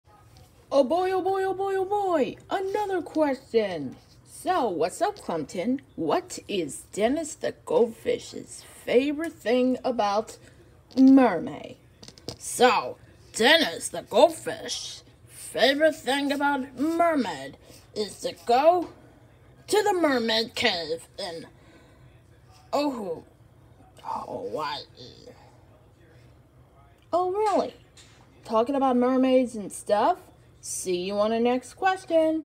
What’s Up Clumpton #368: What is Dennis the Goldfish’s favorite thing about MerMay?
So, Dennis the Goldfish’s favorite thing about MerMay is when Dennis is going to the mermaid cave in Oahu.

Requested by: Kristian SMTV 2022 Cartoon Cartoons (Kristiancastellanos3)
Toono This Weekend created by: Chloe Klingman (ChloeDH1001)